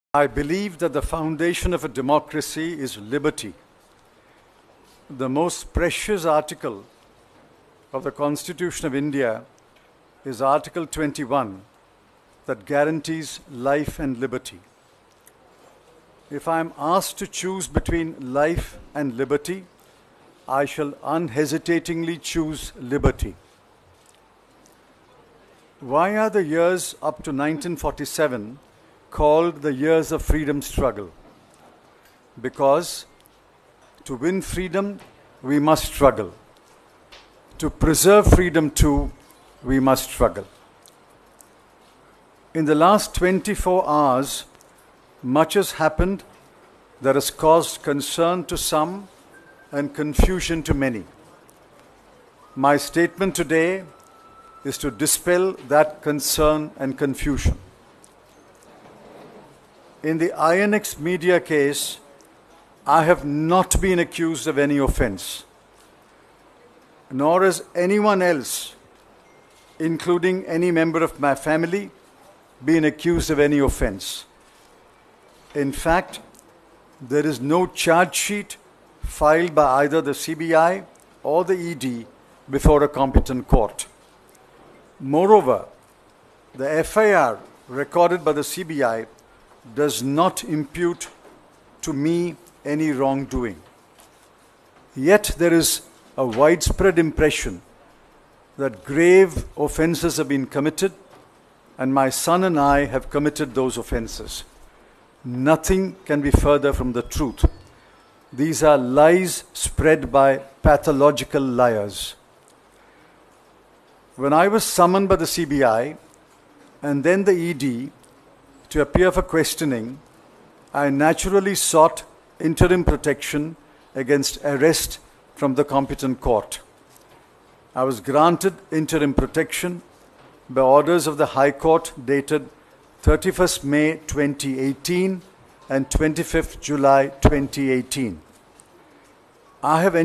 पी चिदंबरम: गिरफ्तारी के ठीक पहले का बयान
पी चिदंबरम को उनके दिल्ली के जोरबाग स्थित घर से जांच एजेंसी ने गिरफ्तार कर लिया है। इसके पहले चिदंबरम कांग्रेस मुख्यालय में प्रेस कॉन्फ्रेंस कर रहे थे।